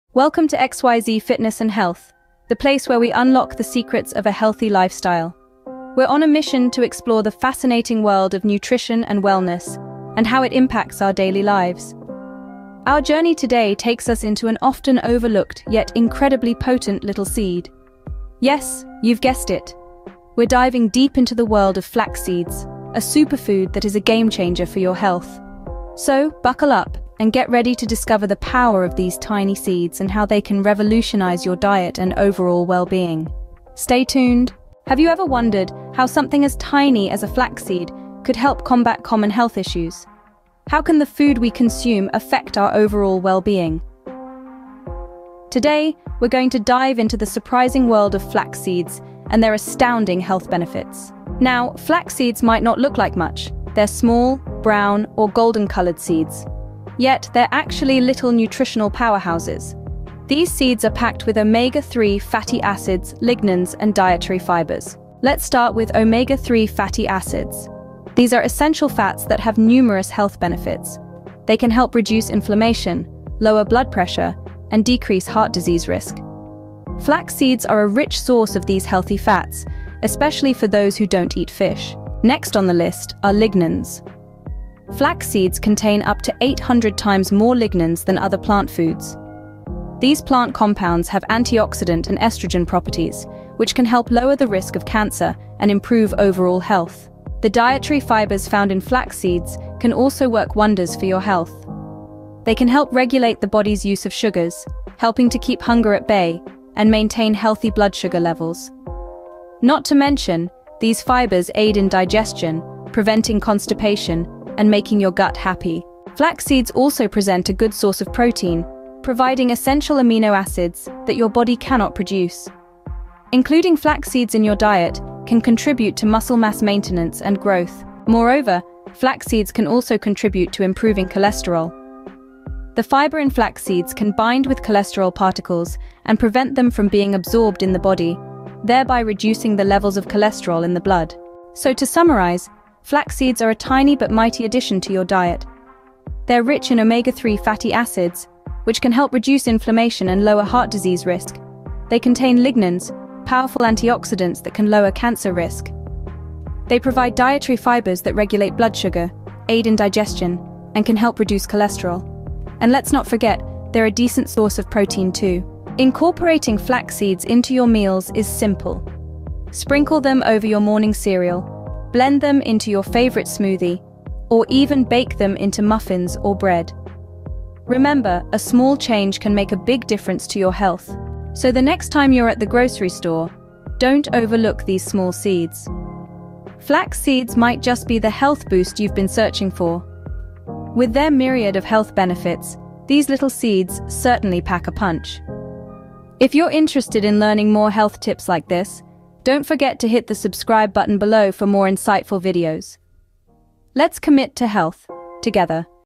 The Miraculous Power of Flaxseeds: Unveiling Their Extraordinary Health Benefits
In this eye-opening video, we delve into the wondrous world of flaxseeds and explore their exceptional health benefits. Prepare to be amazed as we unveil the miraculous power hidden within these tiny seeds. Flaxseeds have gained immense popularity in recent years, and for good reason. Packed with essential nutrients, these super seeds offer a wide array of health advantages that can revolutionize your well-being. First and foremost, flaxseeds are an incredible source of omega-3 fatty acids, boasting more omega-3s than any other plant-based food. Omega-3s play a crucial role in maintaining cardiovascular health, reducing inflammation, and supporting brain function. Incorporating flaxseeds into your diet can help lower the risk of heart disease, enhance cognitive abilities, and promote overall brain health. Additionally, flaxseeds are rich in dietary fiber, aiding in digestion and promoting regular bowel movements. By including these seeds in your meals, you can alleviate constipation, improve gut health, and prevent digestive disorders. The exceptional lignans found in flaxseeds also deserve special mention. Lignans are potent antioxidants that provide a wide range of health benefits, including hormone balance, reduced risk of certain cancers, and improved skin health. These antioxidant properties make flaxseeds a formidable ally in fighting off free radicals and decreasing cellular damage. Furthermore, flaxseeds have been shown to have a positive impact on blood sugar control. They can help stabilize blood sugar levels, making them particularly beneficial for individuals with diabetes or metabolic syndrome. Incorporating flaxseeds into your diet is incredibly simple. They can be easily sprinkled onto cereals, yogurts, smoothies, or incorporated into baked goods like muffins or bread. Get creative and experiment with different ways of including these incredible seeds in your meals to maximize their health benefits. So why wait? Start harnessing the extraordinary health benefits of flaxseeds today! Experience the transformative power that these tiny seeds offer and unlock a healthier you.